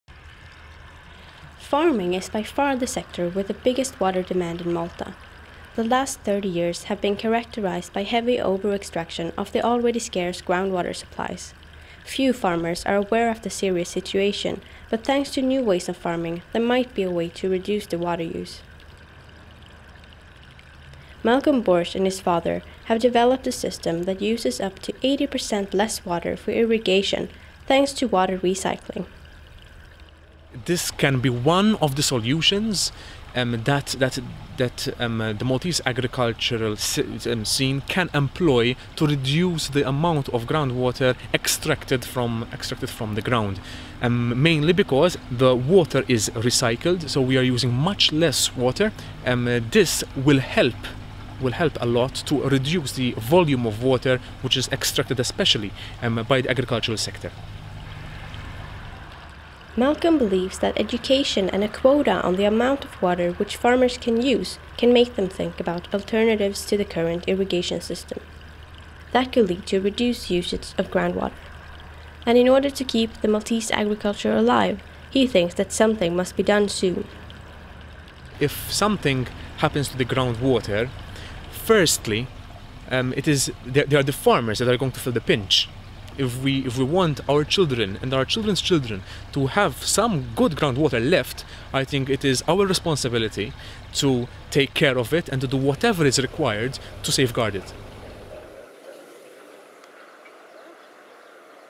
Malta - Water used in farming
Malcolm Borg and his father have developed a system that reduces the amount of groundwater use in farming.